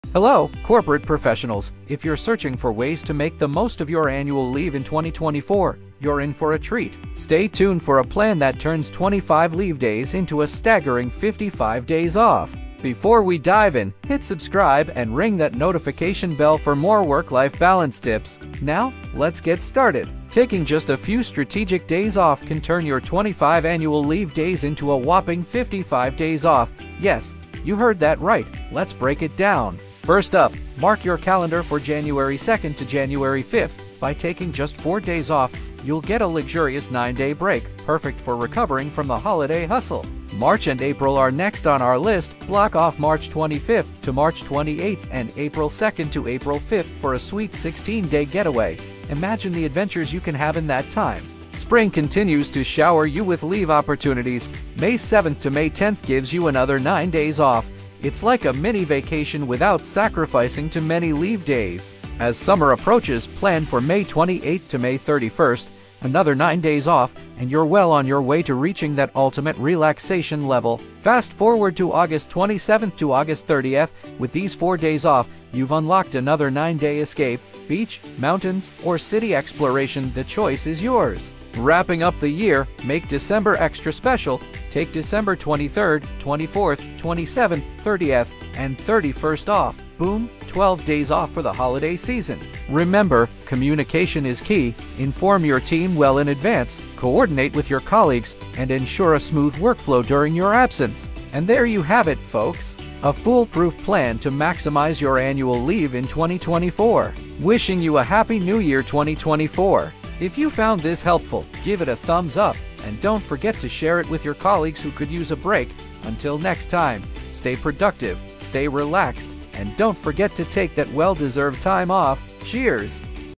Which dates should you book off in 2024 to make 55 days off work in 2024 : explained in 2 minutes 🇬🇧
london travel guide 2023 | The days you need to book off to get 55 days off work in 2024 : explained in 2 minutes 🇬🇧

Whether you're a travel enthusiast, a homebody, or someone looking to strike the perfect work-life balance, we've got tips and tricks to help you maximize those precious days off.

In this video I have explained how to get 55 days off by using the 25 days annual leaves.

If you work in a Monday- Friday job in corporates this serves as a guidance.

Take January 2nd- January 5th for 9 days off

Take March 25th - March 28th & April 2nd - April 5th for 16 days off

Take May 7th May - 10th for 9 days off

Take May 28th May - 31st for 9 days off

Take August 27th - August 30th for 9 days off

Take December 23rd, 24th, 27th, 30th and 31st for 12 days off

25 ANNUAL LEAVE DAYS = 55 DAYS OFF

📍United Kingdom

Forward this video to someone who needs to maximise their annual leave in 2024. Thank me later 🫶🏾